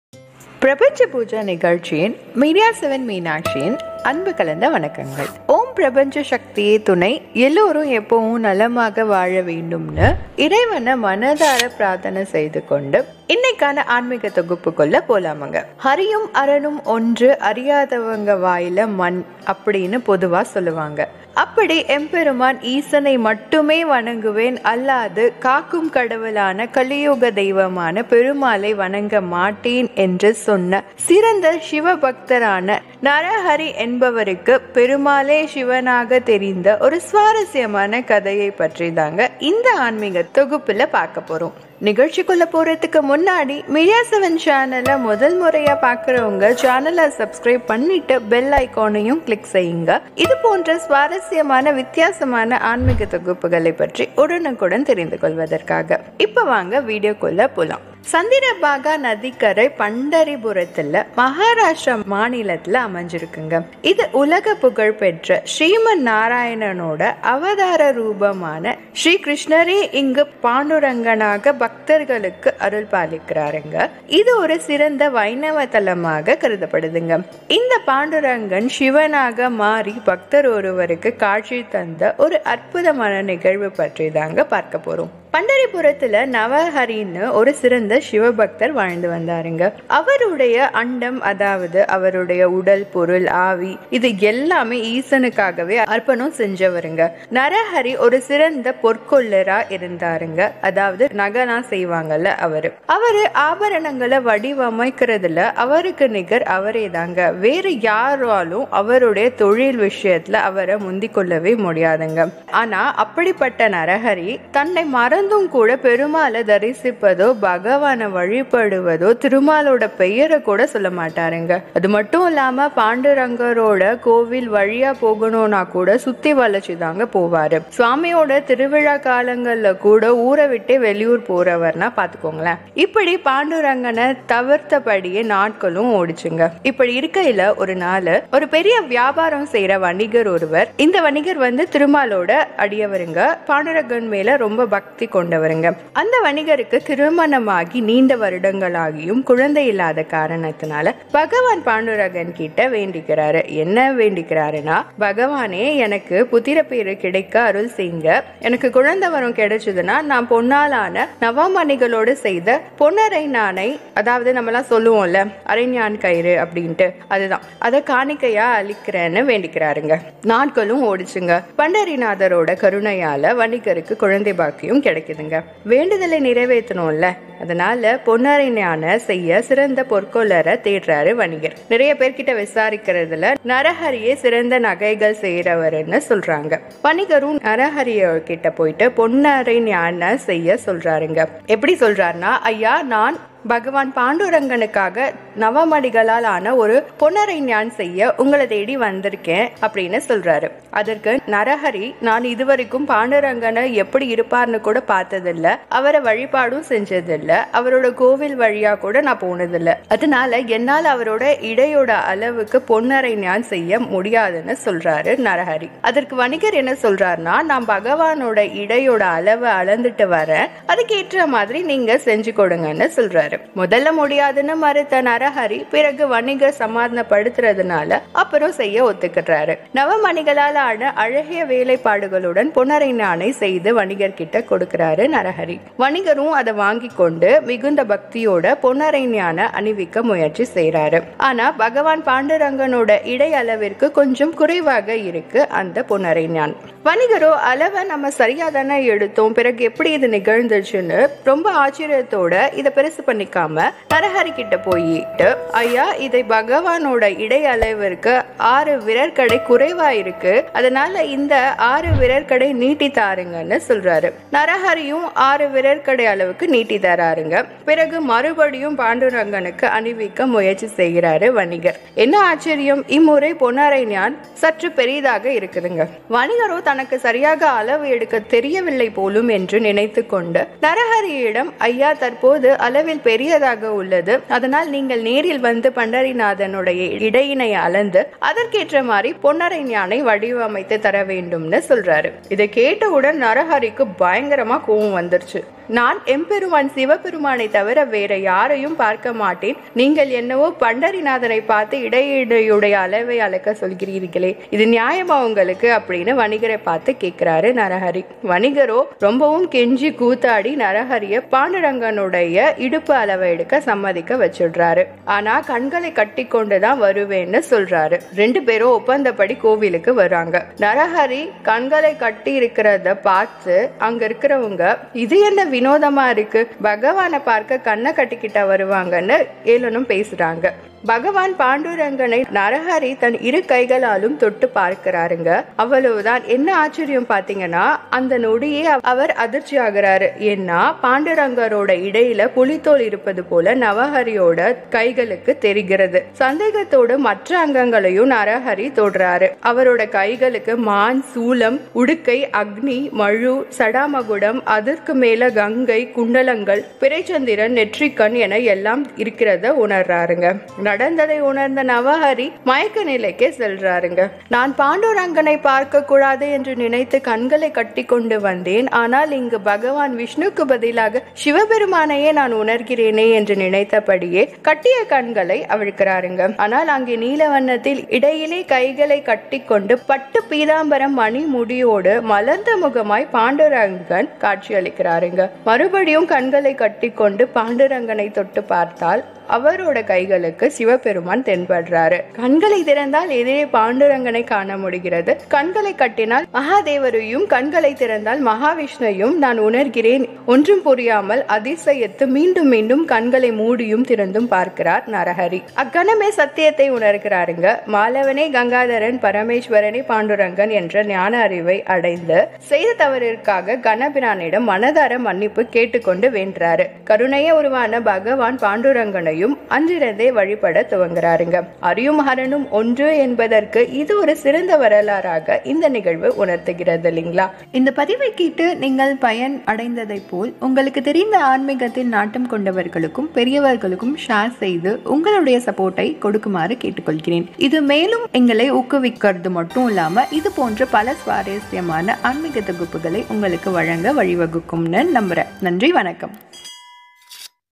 அரியும் சிவனும் ஒன்று அறியாதவர் வாயில் மண்ணு | நரஹரி கதை | சிவபெருமானின் பக்தர் நரஹரி வரலாற்று கதை
அரியும் அரணும் ஒன்று அறியாதவர் வாயில் மண்ணு